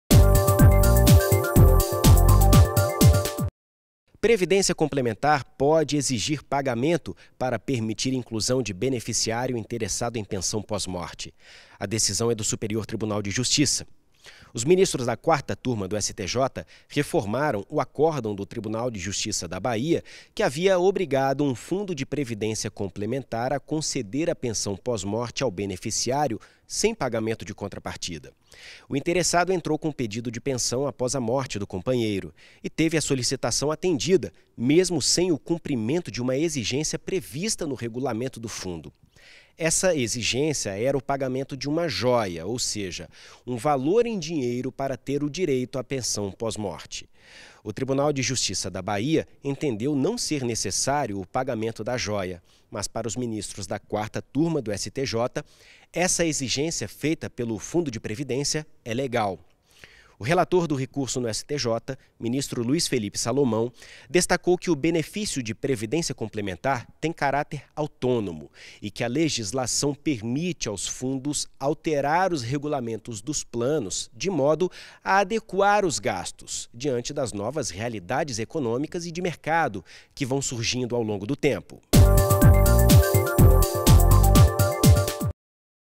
Clique STJ - Morte Previdência (18/02/2019)
Previdência complementar pode exigir pagamento para permitir inclusão de beneficiário interessado em pensão pós-morte. A decisão é do Superior Tribunal de Justiça.